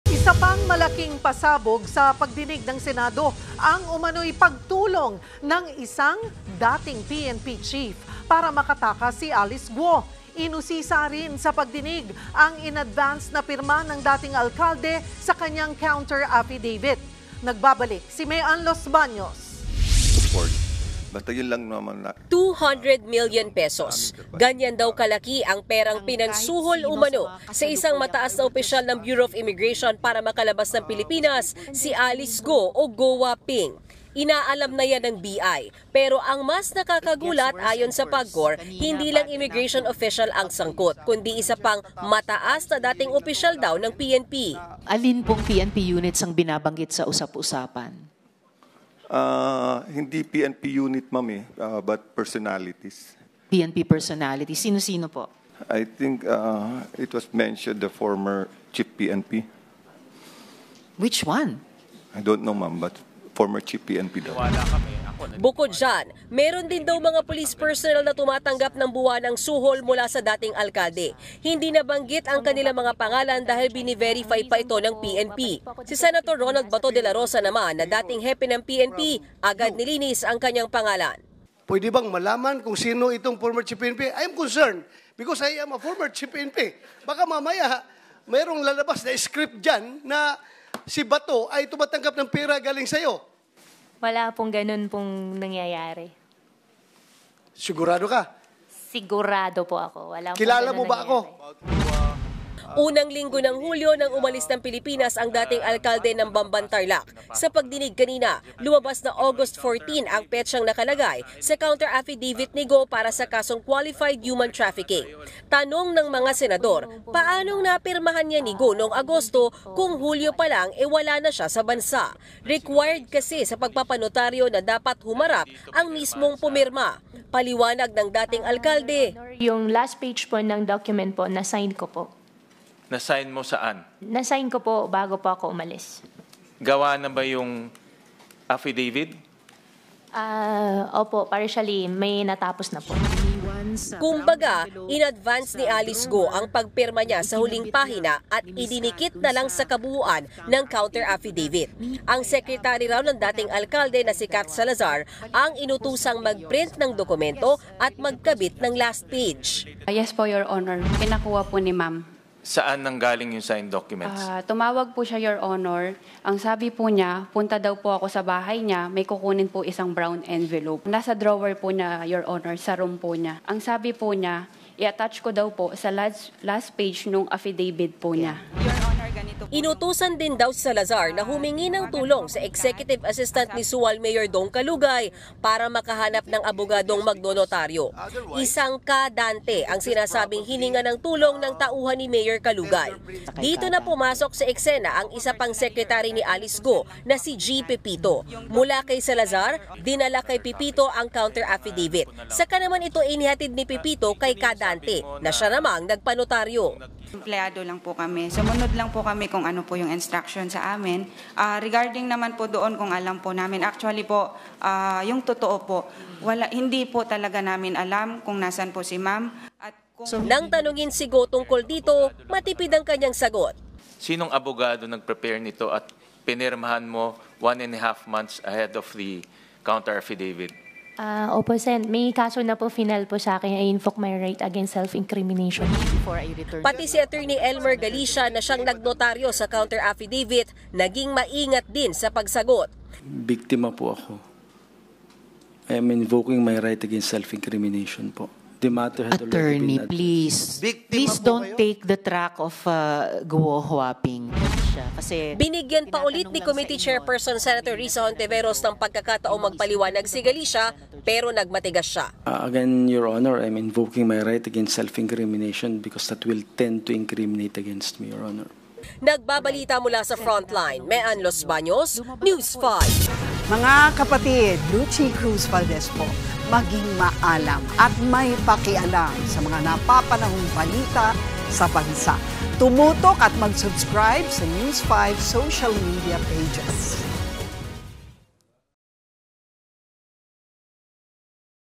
Mataas na opisyal ng BI at isang dating PNP chief, tumulong umano sa pagtakas ni Alice Guo
FrontlinePilipinas | Isa pang malaking pasabog sa pagdinig ng Senado ang umano’y pagtulong ng isang dating Philippine National Police PNP chief para makatakas si Alice Guo.

Inusisa rin sa pagdinig ang in-advance na pirma ng dating alkalde sa kanyang counter-affidavit. News5 | via Maeanne Los Baños-Oroceo